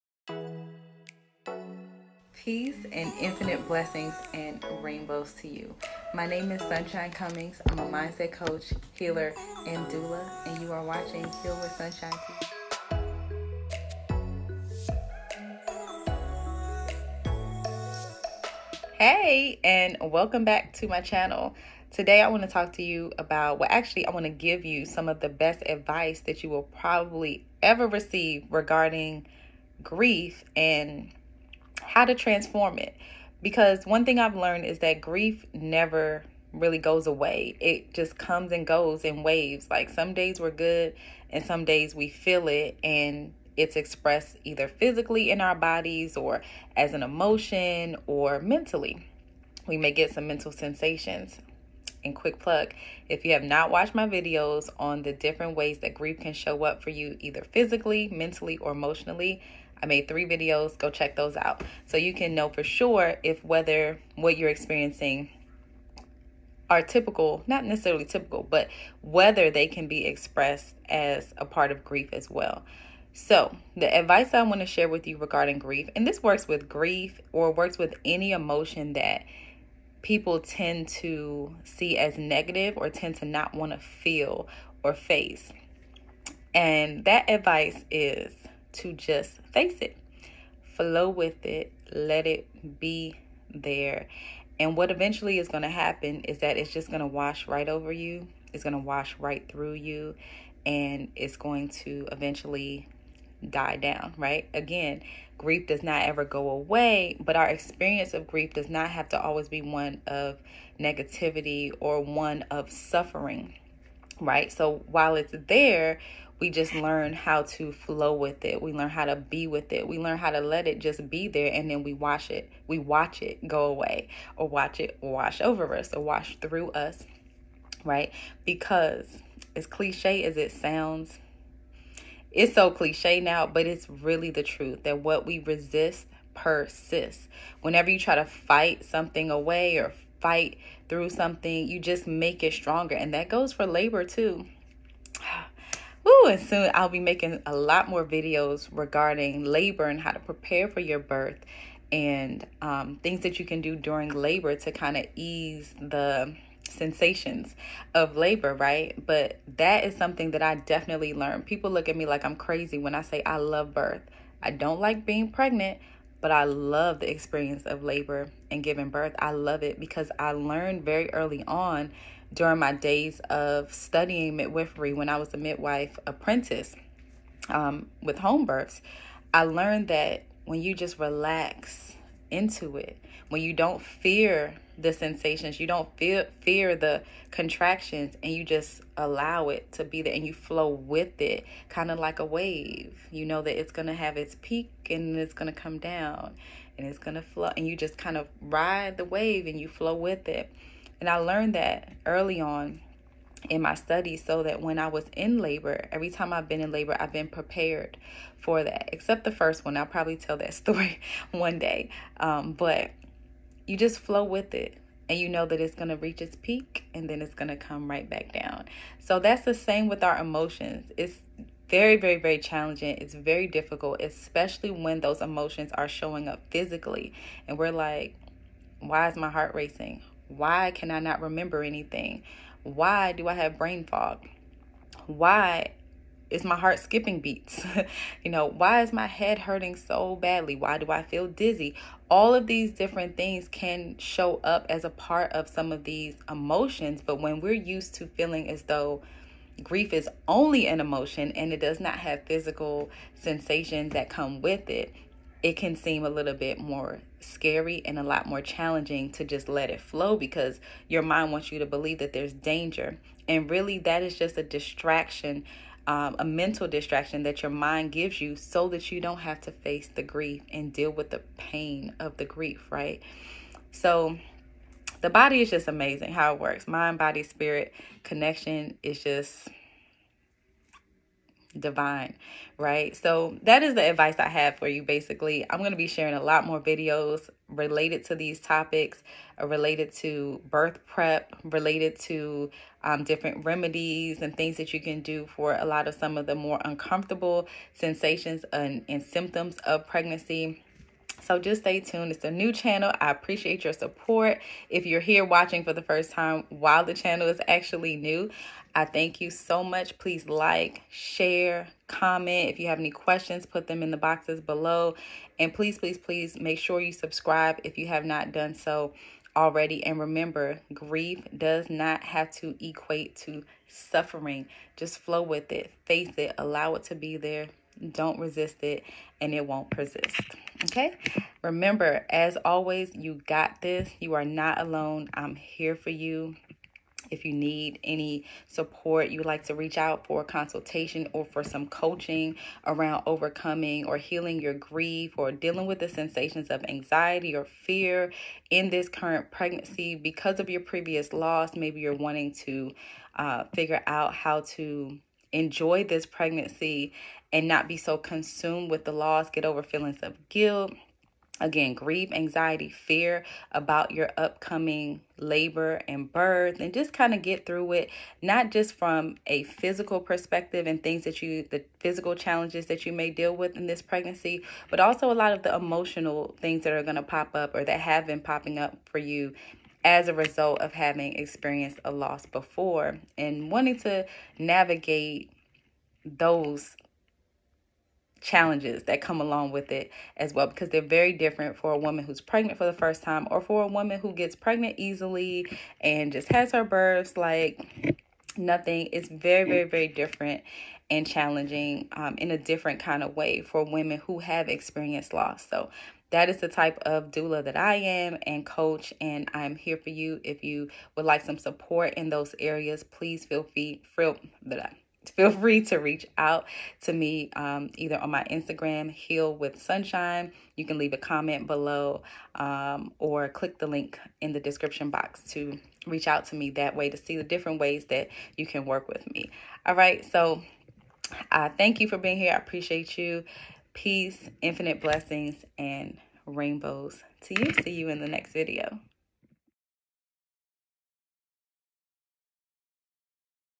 Whenever Grief Over Your Miscarriage Pops Up…DO THIS!
Watch this video for advice on what to do whenever grief over your pregnancy loss/miscarriage pops up!